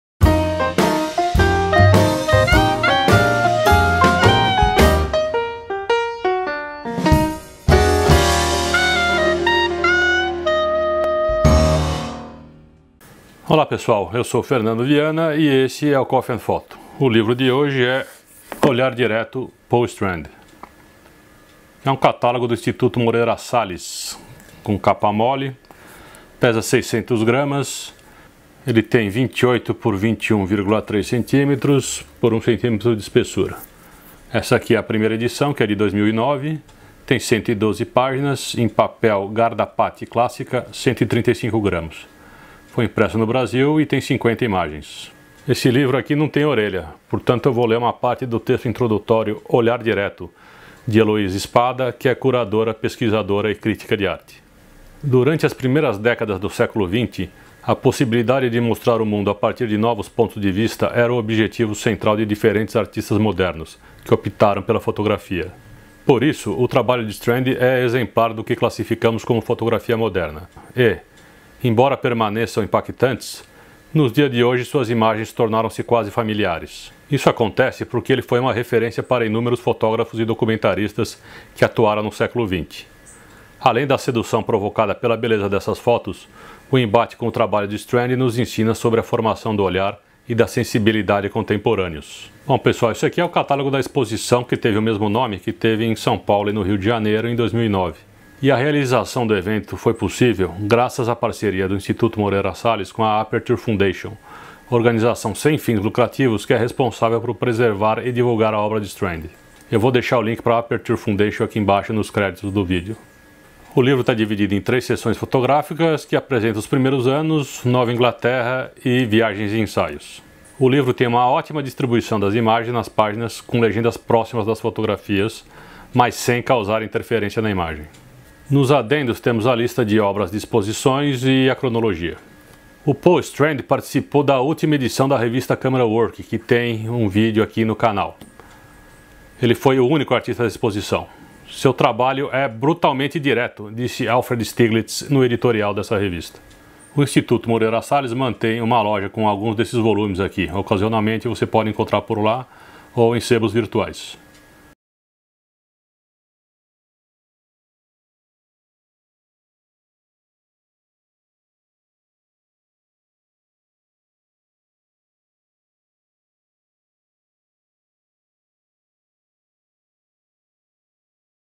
Coffee and Photo 07 - Paul Strand - Olhar direto - por Fernando Vianna
Análise do livro Paul Strand - Olhar Direto - Editado pelo IMS

Revisão de texto - Luiza Thebas - @luizathebas